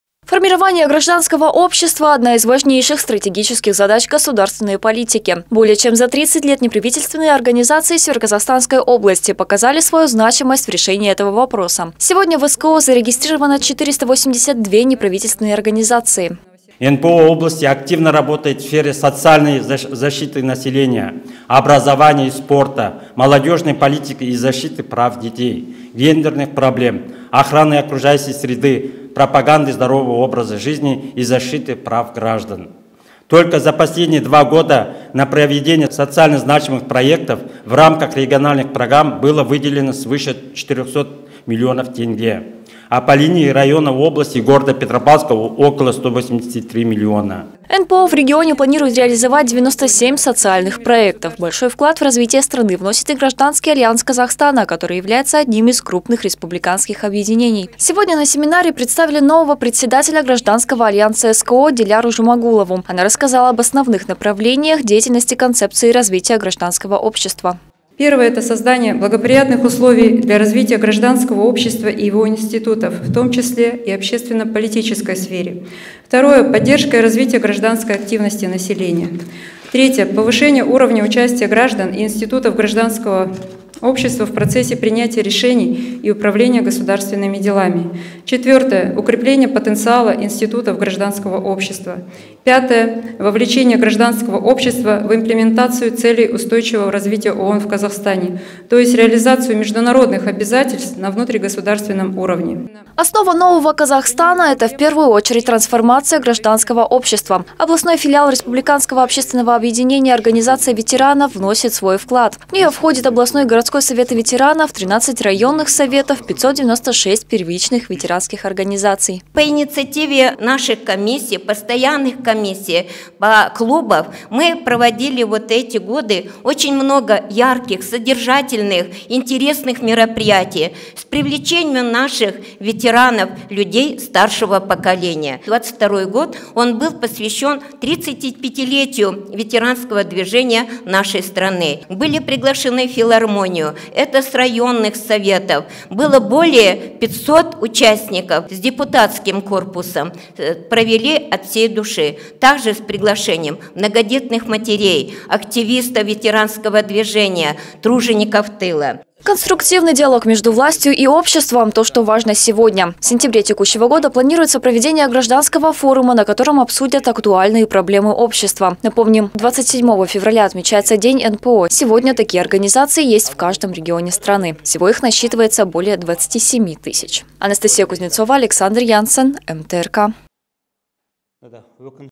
Роль НПО в формировании гражданского общества на примере СКО
Формирование гражданского общества - одна из важнейших стратегических задач государственной политики. Более чем за 30 лет неправительственные организации СКО показали свою значимость в решении этого вопроса. Сегодня в СКО зарегистрировано 482 неправительственные организации.